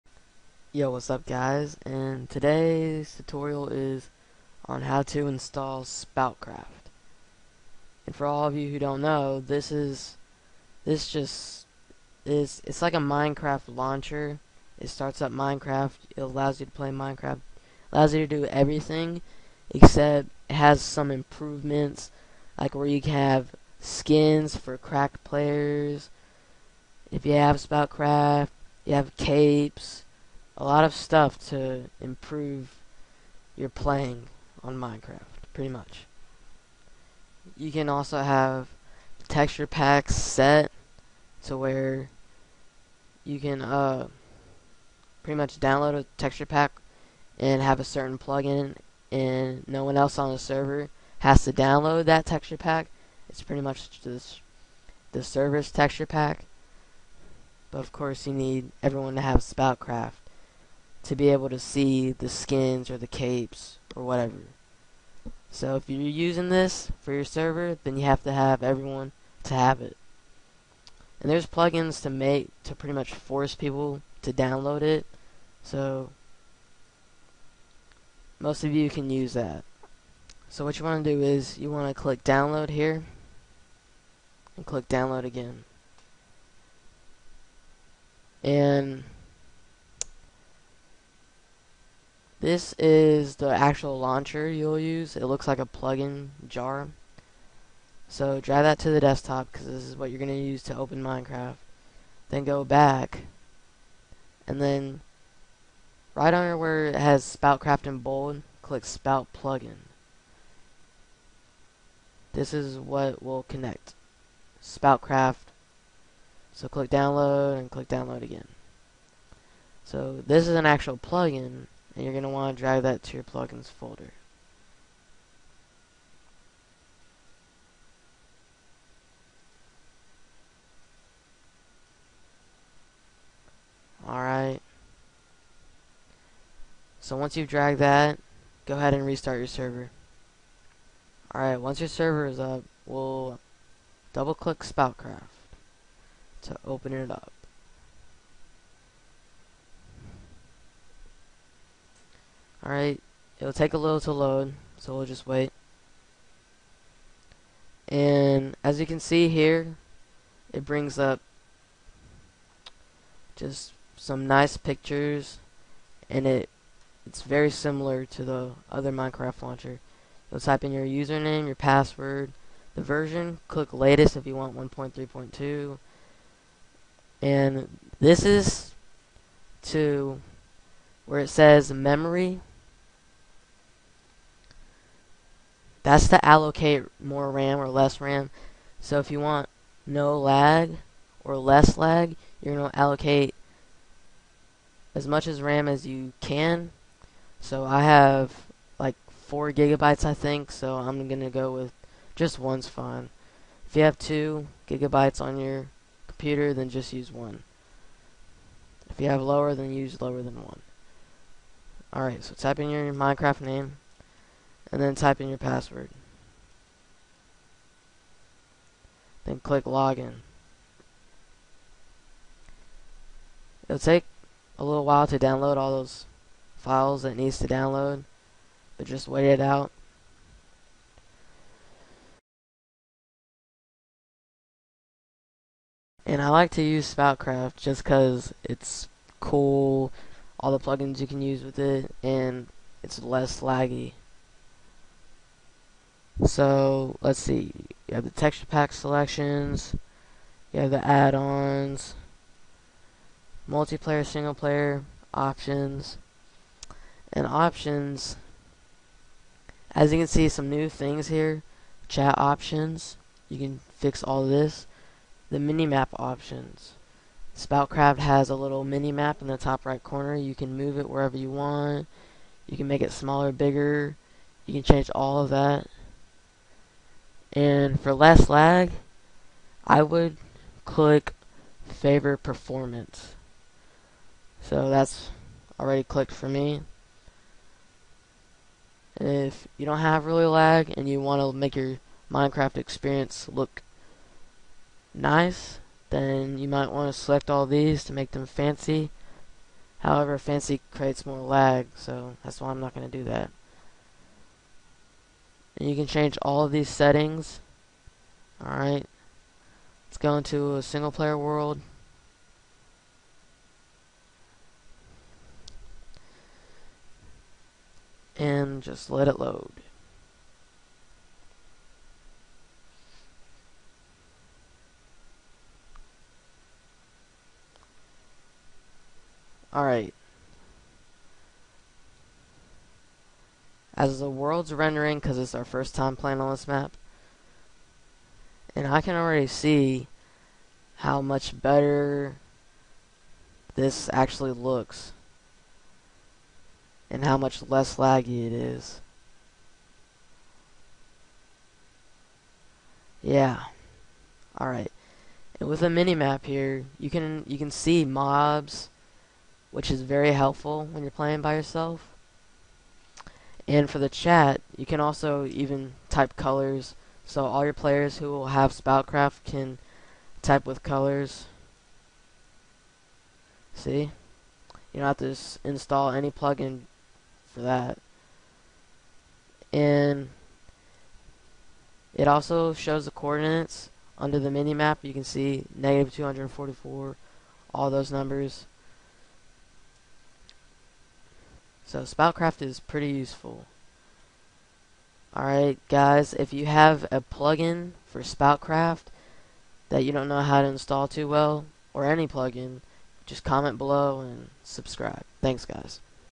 How to install Spout for your Server! (1.3.2)
In this tutorial I will show you how to use Spout with the SpoutPlugin installed on your server.